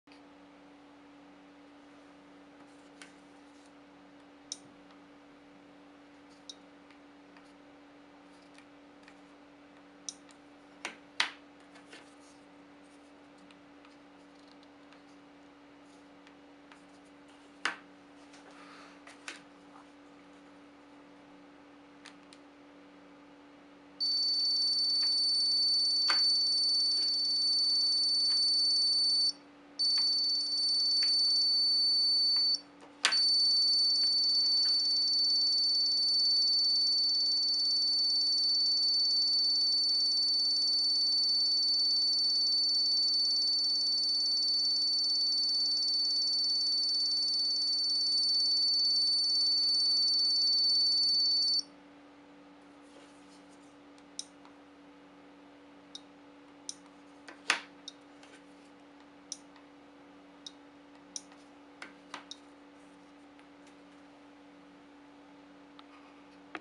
Programmazione telecomandi Aprimatic - Attenzione al suono!!!
Programmazione telecomandi Aprimatic
1 - Premere contemporaneamente tutti e 4 i pulsanti sul telecomando precedentemente registrato nella memoria del ricevitore e attendere che suoni un segnale acustico prolungato che indica l'inizio della procedura di autoapprendimento.
2 - Premere contemporaneamente tutti e 4 i pulsanti sul nuovo telecomando, il segnale acustico si interromperà e riprenderà di nuovo.
3 - Premere qualsiasi pulsante sul nuovo telecomando. Il segnale acustico si spegnerà per un breve periodo e si accenderà di nuovo, confermando la registrazione delle informazioni del telecomando nella memoria del ricevitore radio.
4 - Durante il segnale acustico (circa 25 secondi), è possibile continuare la procedura di autoapprendimento di nuovi telecomandi ripetendo i passaggi 2 e 3.
5 - Successivamente, è possibile avviare nuovamente la procedura di autoapprendimento dei telecomandi premendo contemporaneamente tutti i pulsanti su qualsiasi telecomando già registrato nella memoria. Si sentirà un segnale acustico e sarà possibile aggiungere altri telecomandi alla memoria ripetendo i passaggi 2 e 3.